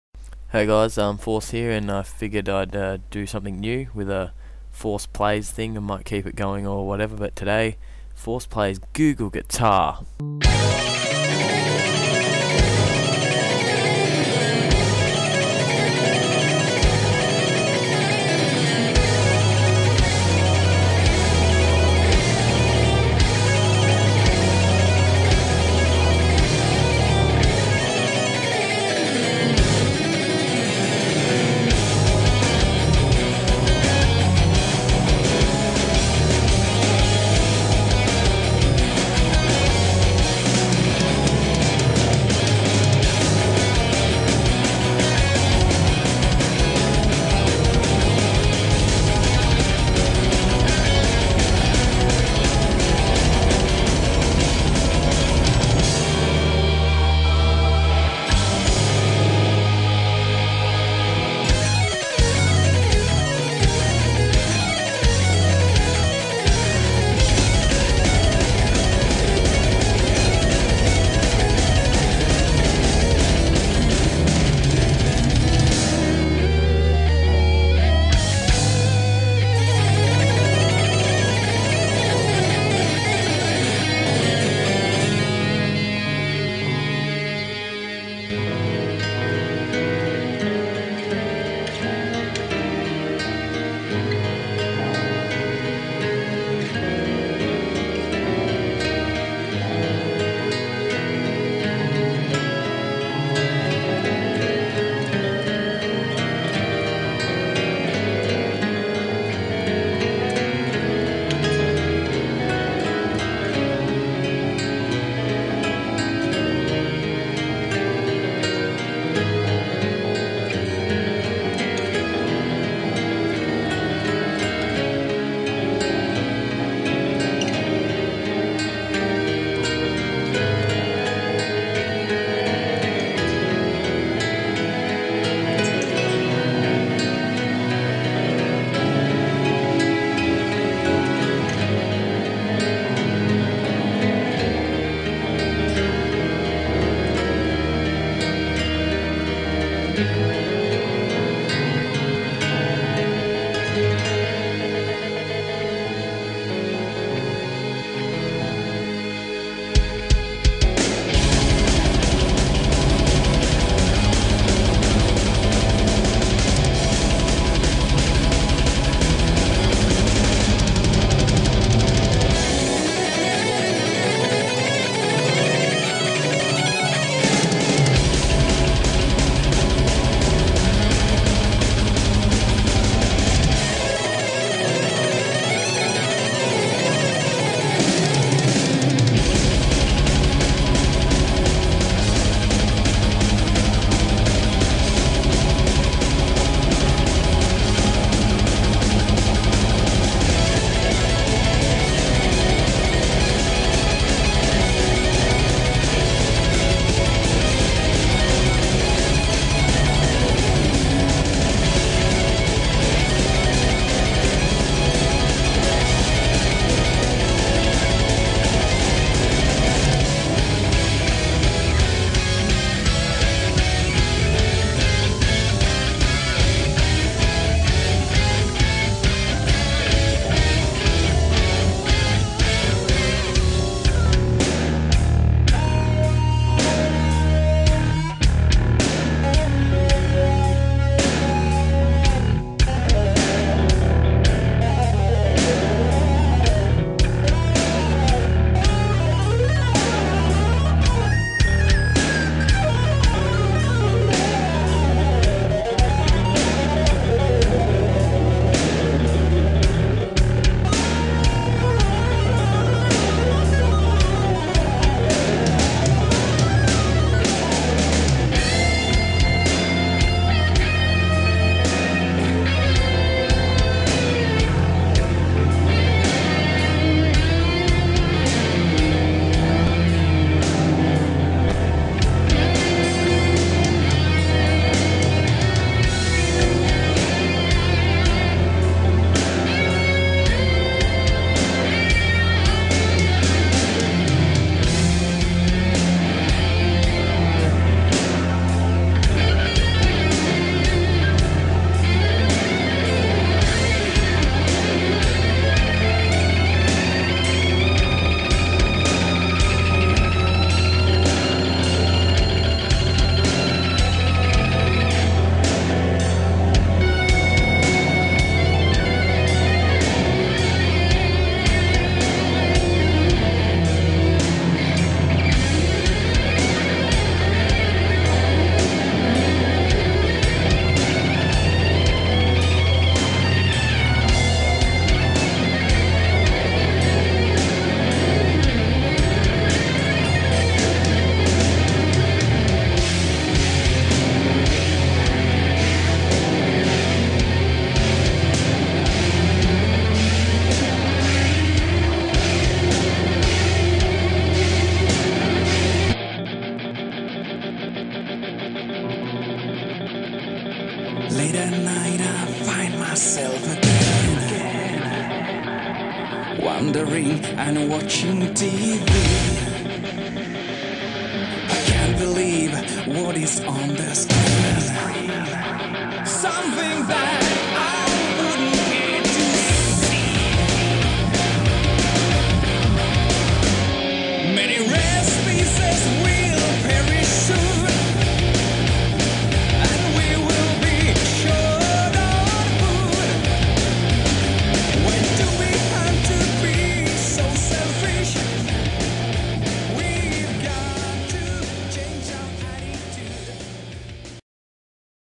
Force Plays - Google Guitar!
This is the first "episode" of my new range of videos, me being fucked to play games, this one does not have any commentary, but it's aight.

All rights to these songs are owed by Stratovarius